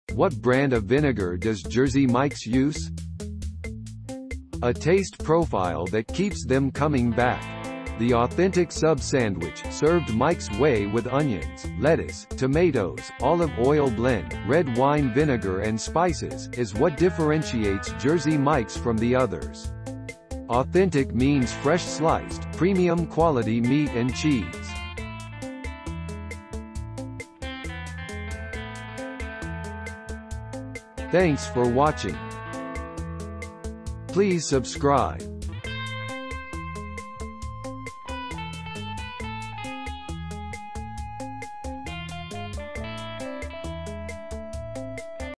What brand of vinegar does Jersey Mike's use?
A Taste Profile That Keeps Them Coming Back. The authentic sub sandwich, served Mike's Way with onions, lettuce, tomatoes, olive oil blend, red wine vinegar and spices, is what differentiates Jersey Mike's from the others. Authentic means fresh sliced, premium quality meat and cheese.